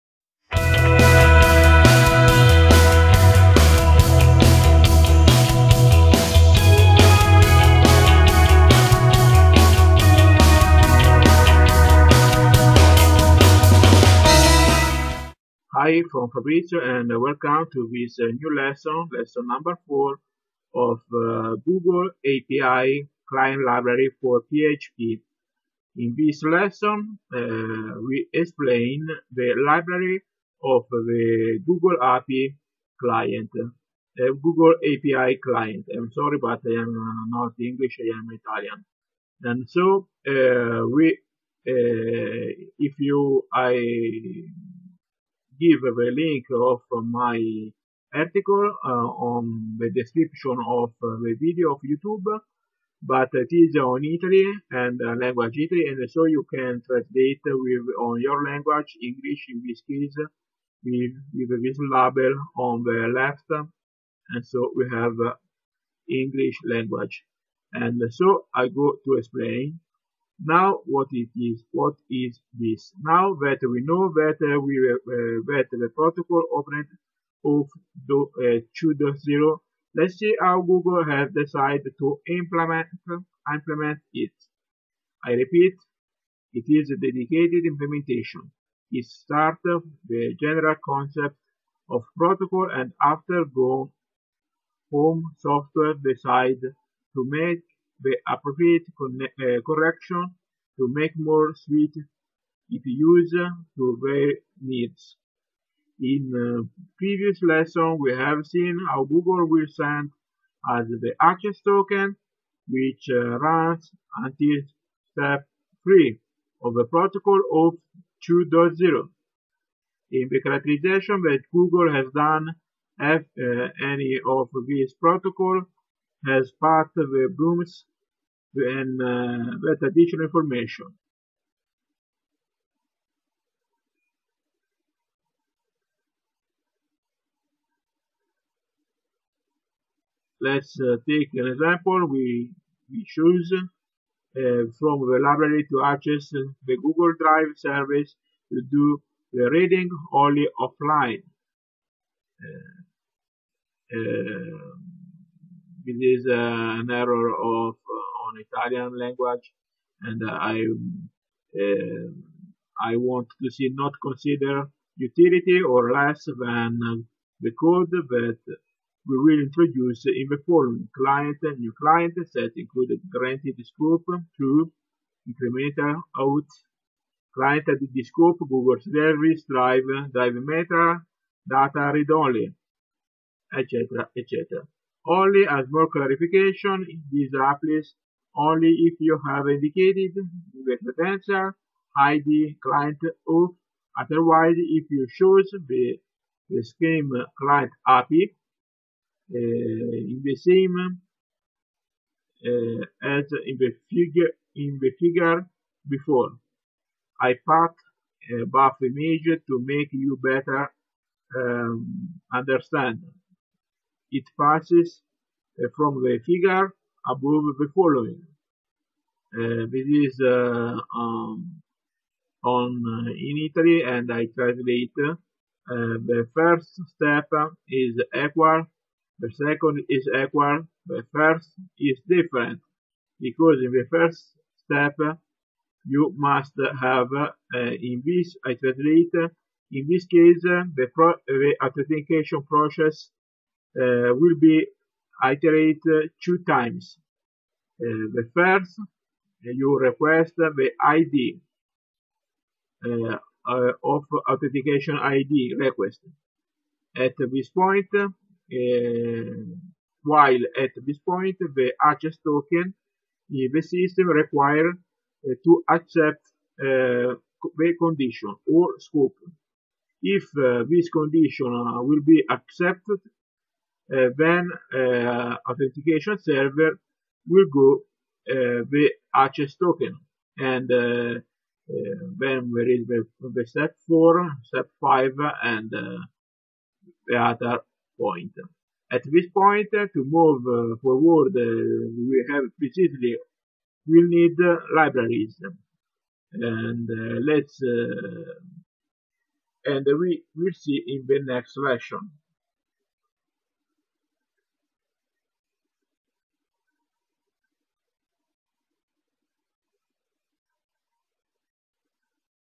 Google Api Key PHP Lesson 4 The Library
Tutorial about Google Api Key PHP for system Authentication In this new lesson we speak about the Google Library to choice a service of Oauth